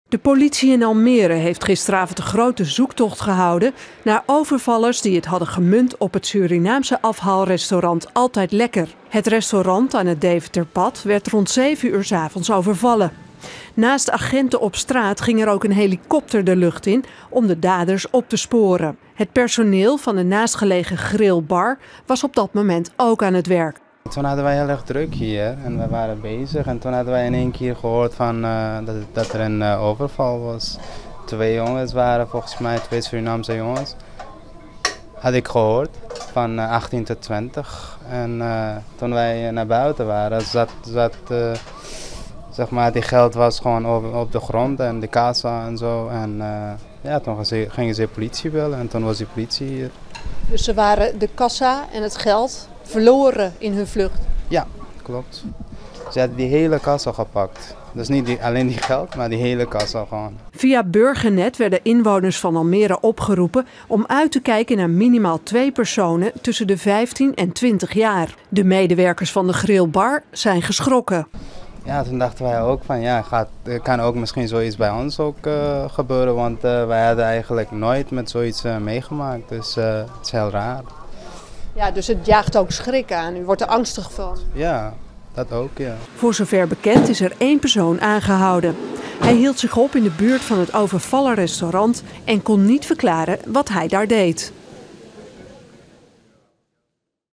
Overval op restaurant, politie kamt Stedenwijk uit
De politie zocht maandagavond naar de daders van een overval in de Almeerse Stedenwijk.

De overval werd rond 19:00 uur gepleegd op Surinaams afhaalrestaurant 'Altijd Lekker Almere' aan het Deventerpad. De politie zocht met een helikopter boven de Stedenwijk naar de daders.

Via Burgernet riep de politie Almeerders op om uit te kijken naar minimaal twee personen in de leeftijd van 15 tot 20 jaar. Agenten hebben in de buurt van het restaurant een persoon aangehouden omdat hij niet kon verklaren wat hij daar deed.